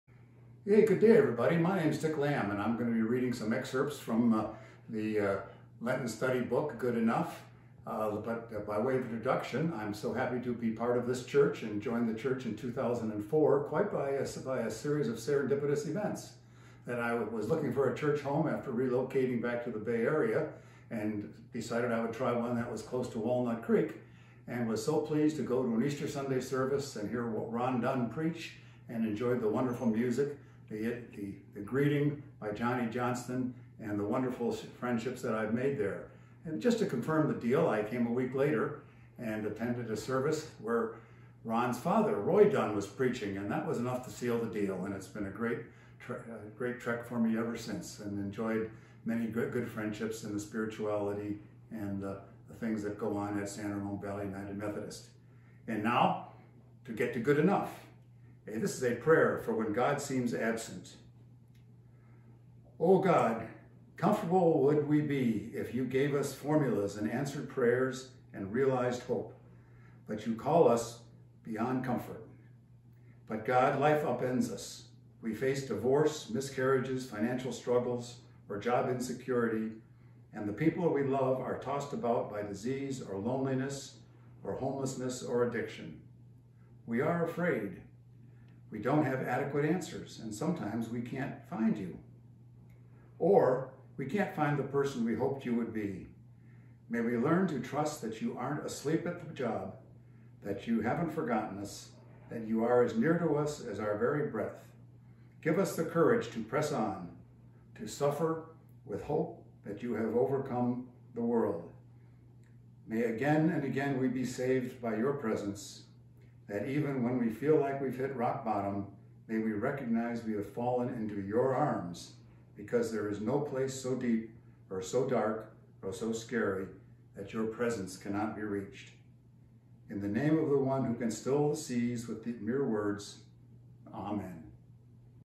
Lent Devotional: A Prayer for When God Seems Absent
Dick Lam reads an excerpt from Good Enough: 40ish Devotionals for a Life of Imperfection by Kate Bowler.